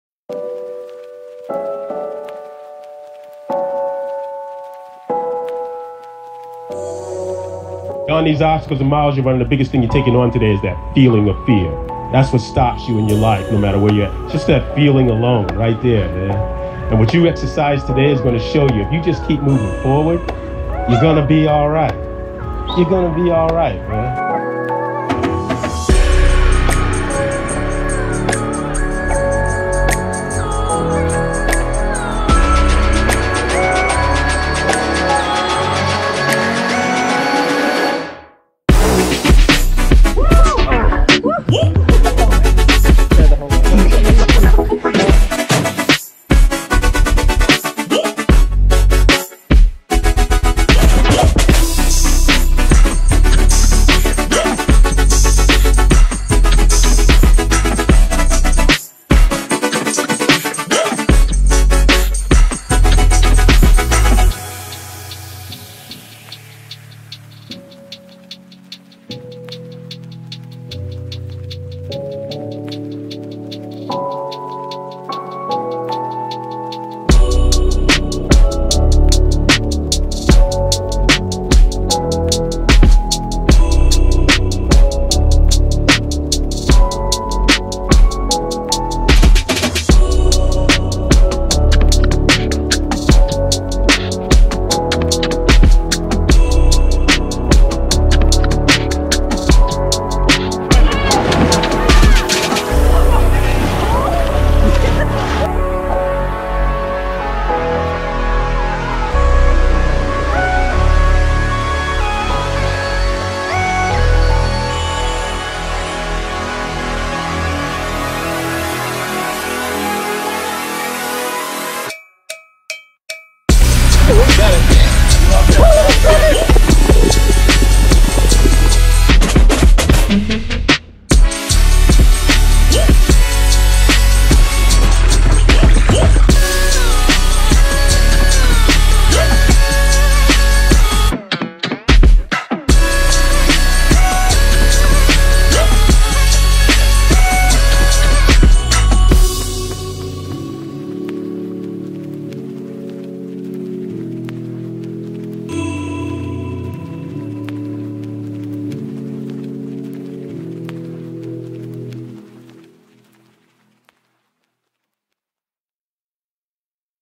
Tough Mudder Virginia 2019 (Motivational)
Tough Mudder Virginia 2019. Classic 10 Miles, 25 Obstacles.

"Don't let fear keep you from moving forward."